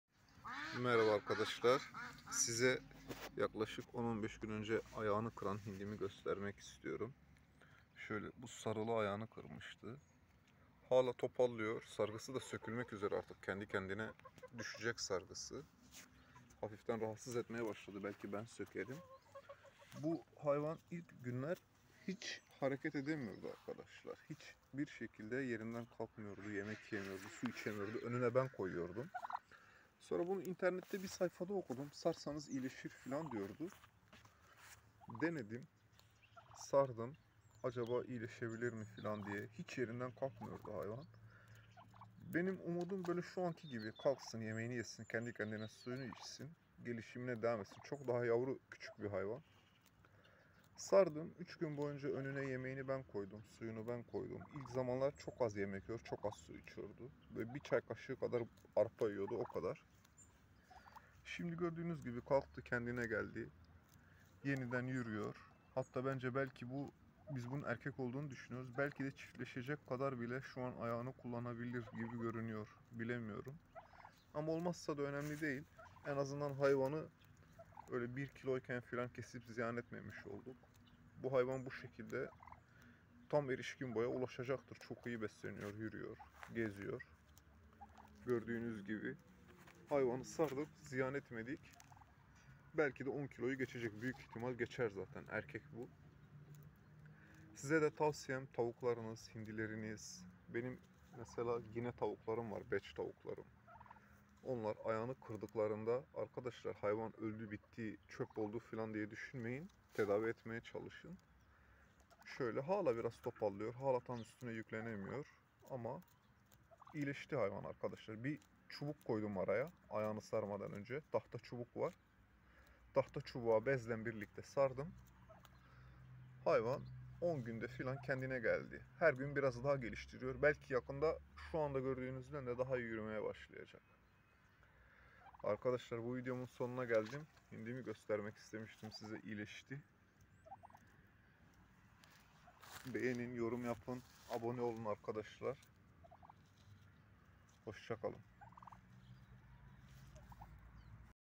Hindi ve Tavukların ayak kırıkları nasıl tedavi edilir
ayağı kırılan hindi tavuk gibi kümeshayvanları tedavisi için bu videomu mutlaka izleyin.